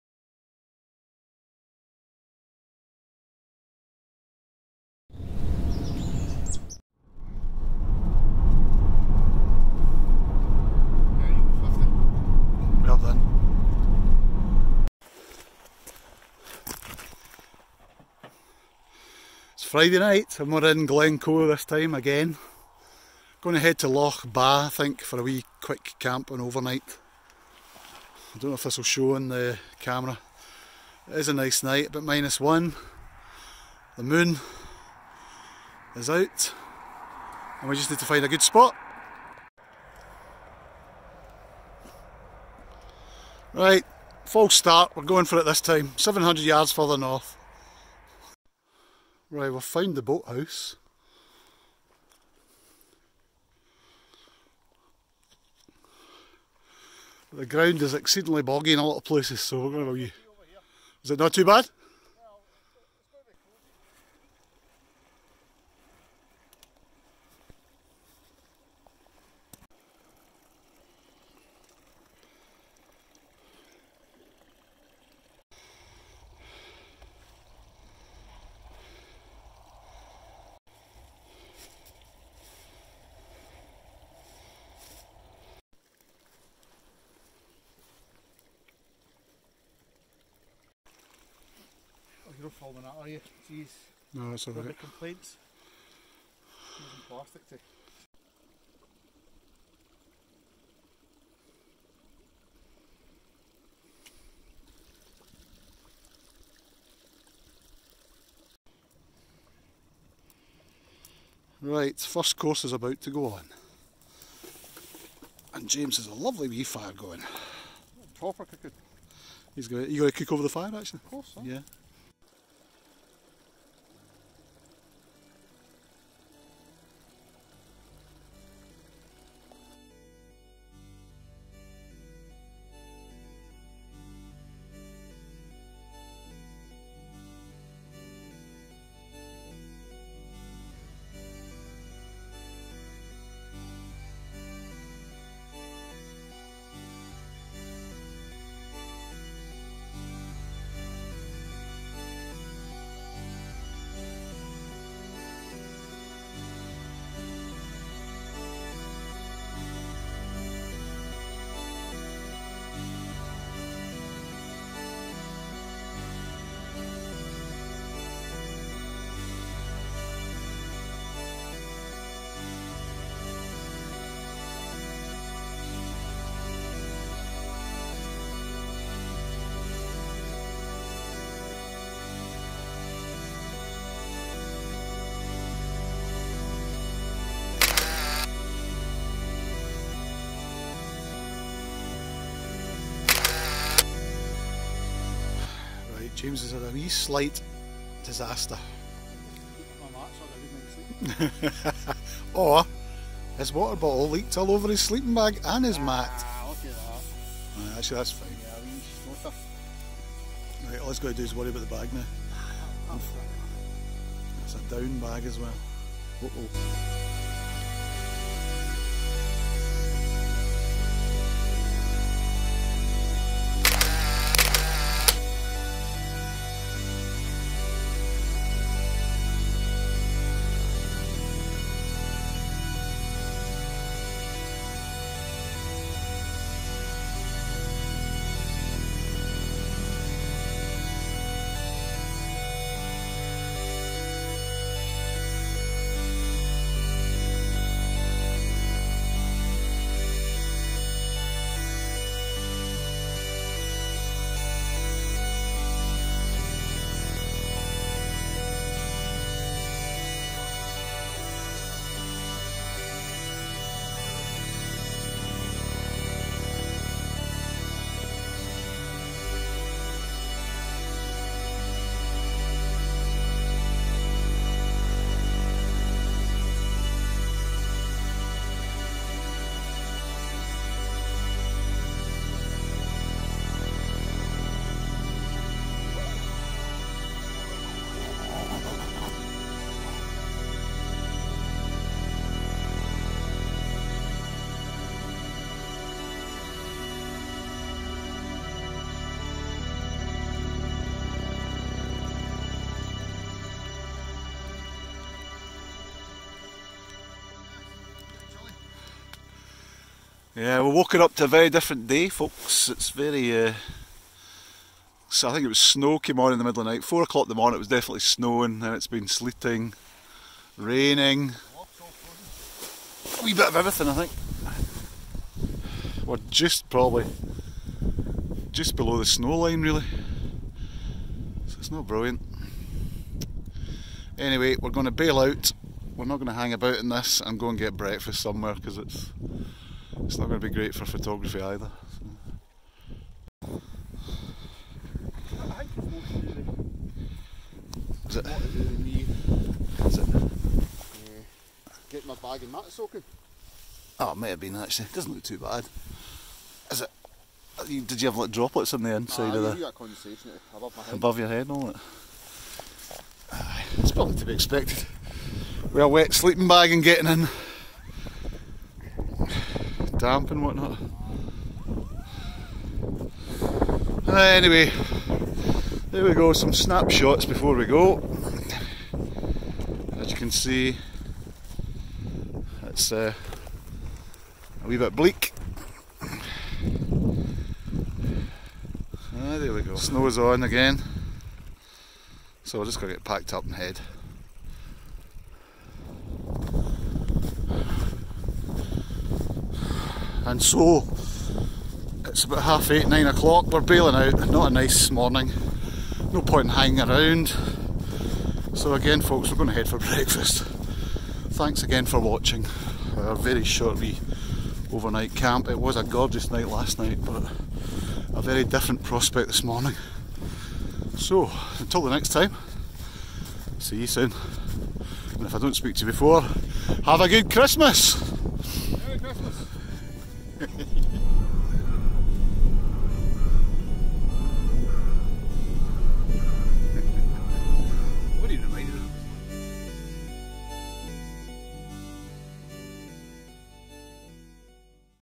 #145 Loch Ba Wildcamp, Cooking & Fire (LNT) - Rain stops play - Rab Mk3 Shelter
A very quick wild camp on Loch Ba, Glencoe and a first chance to try the Rab Mk3 eVent tent out. unfortunately the forecast snow turned out to be rain so Saturday was a wash out. Note if you have a fire Leave No Trace.  Oh and Jim wets himself in his MSR Access 1 :-)